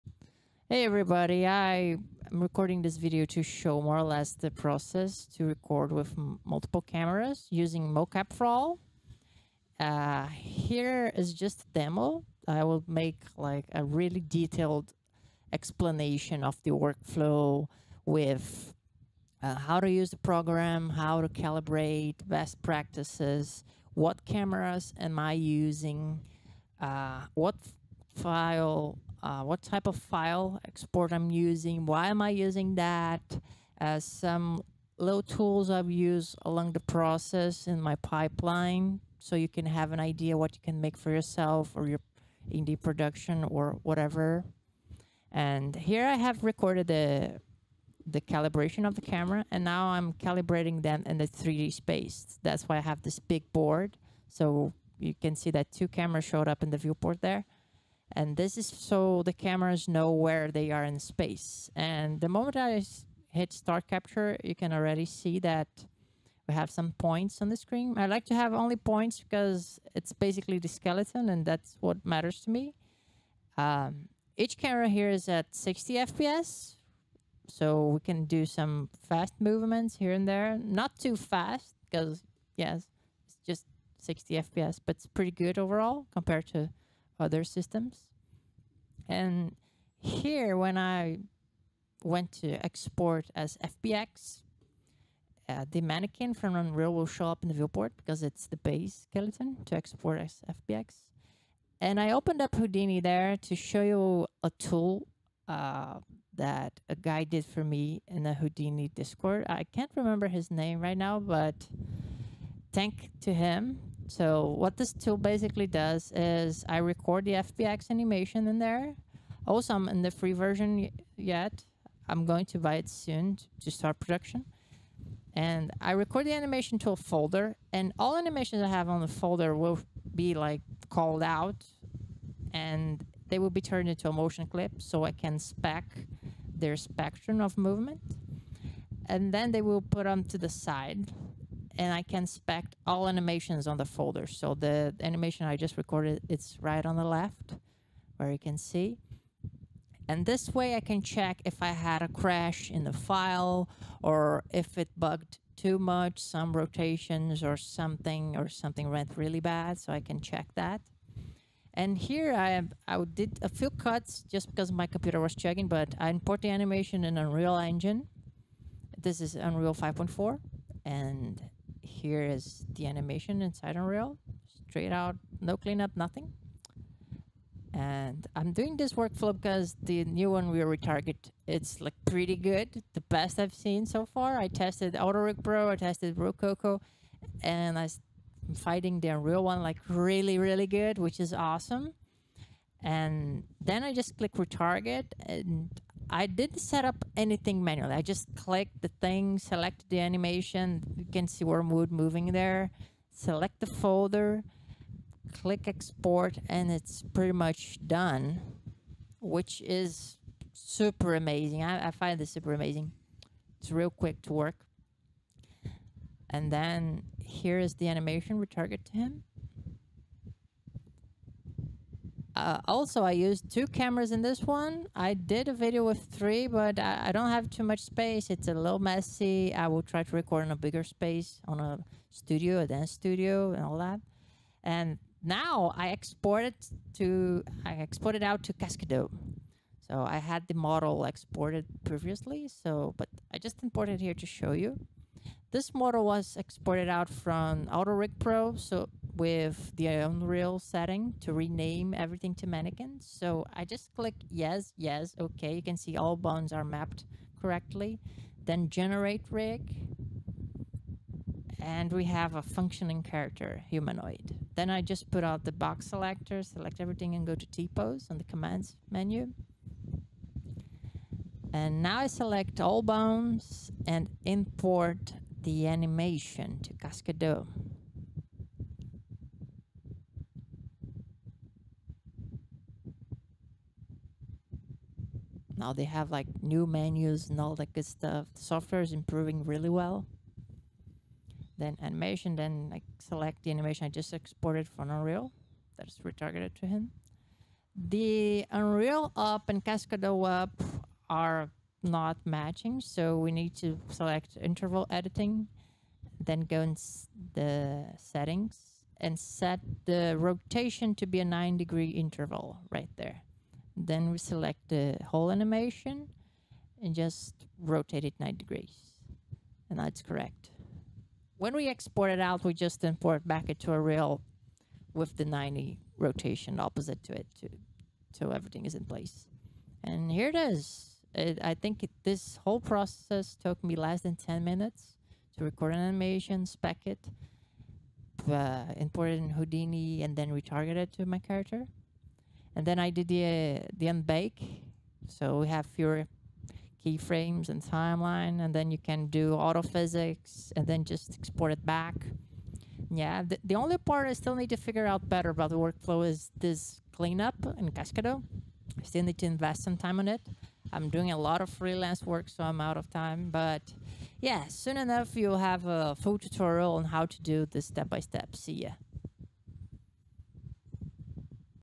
Multi Camera Markeless Motion Capture Workflow Demo UE 5.4
here is a quick demo of my whole workflow for multicam mocap